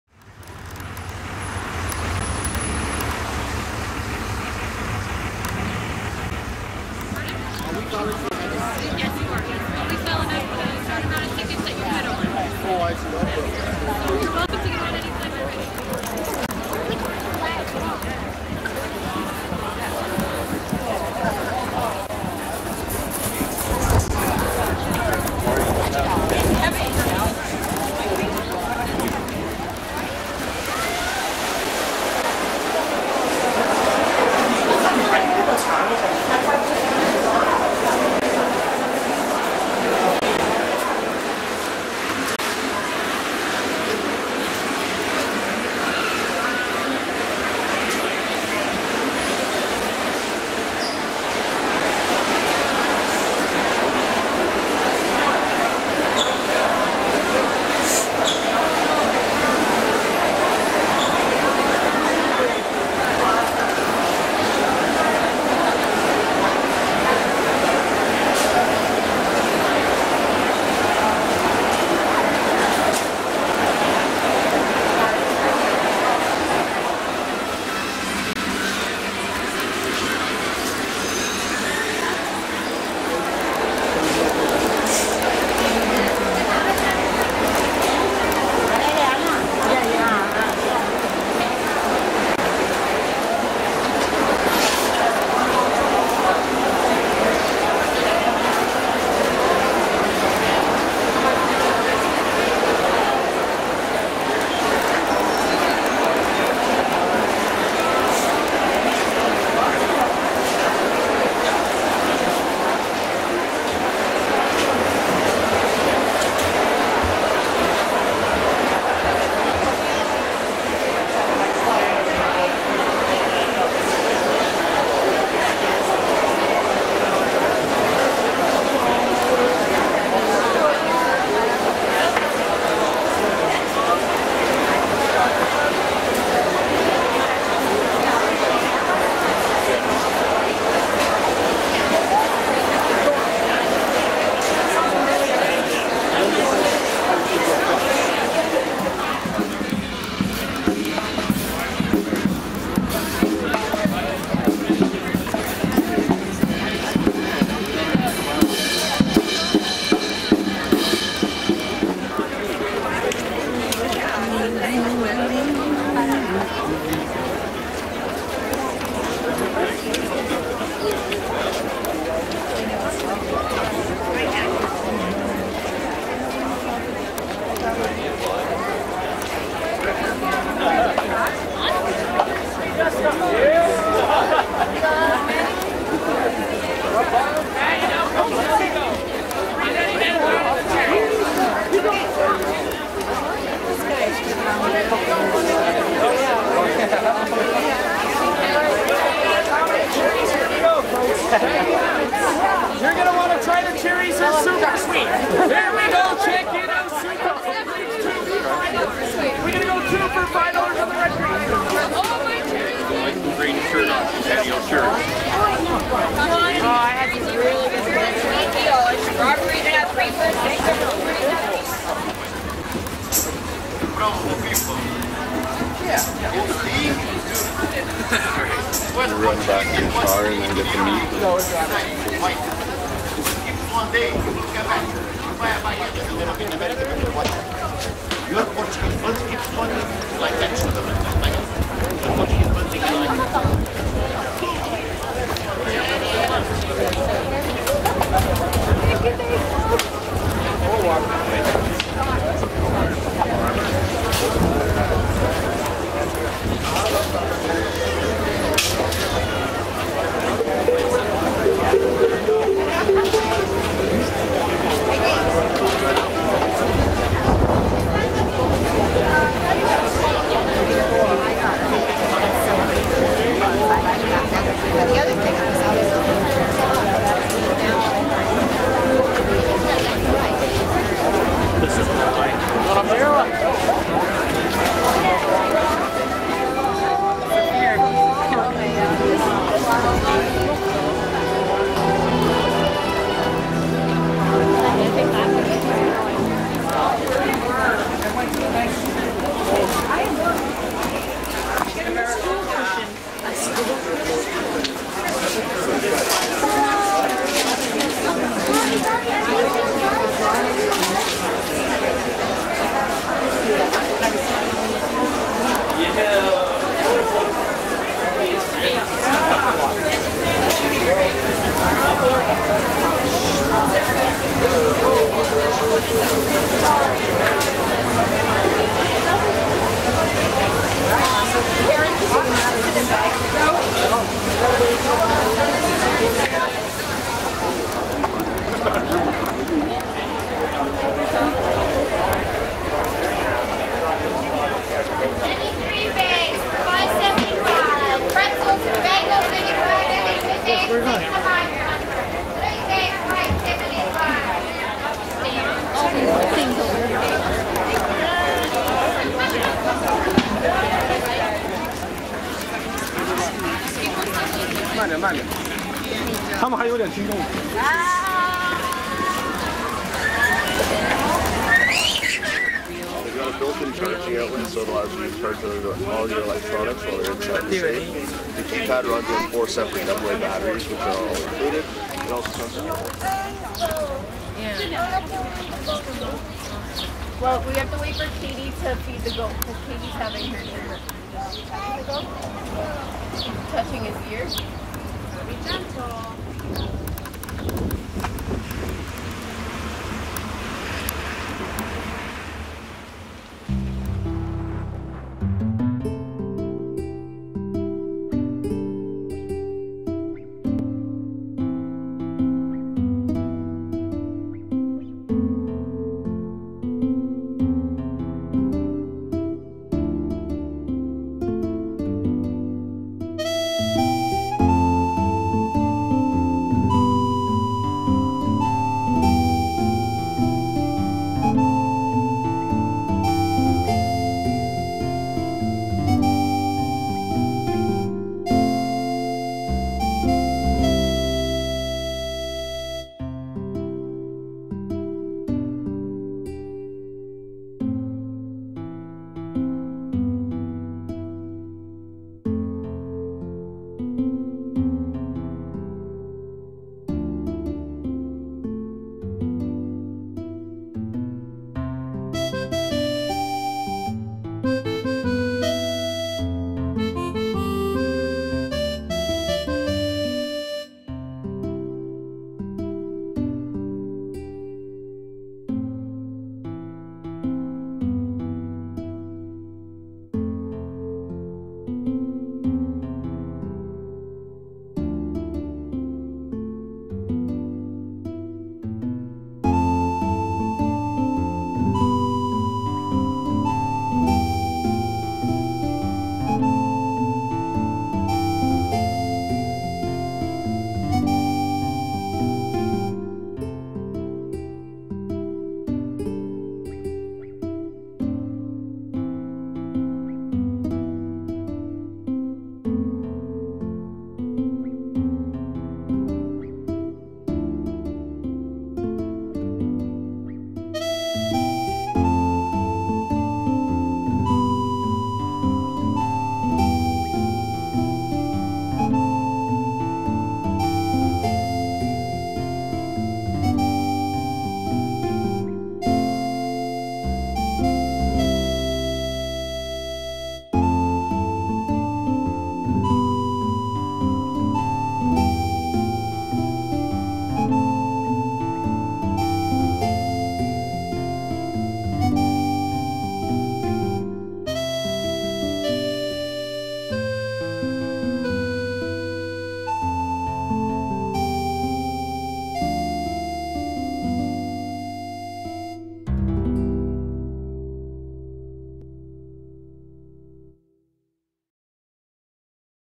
St. Jacobs Country Ontario Canada
the new building at the Market finally opened but not 100% occupancy
yet. Then a drive down King St. in the village of St. Jacobs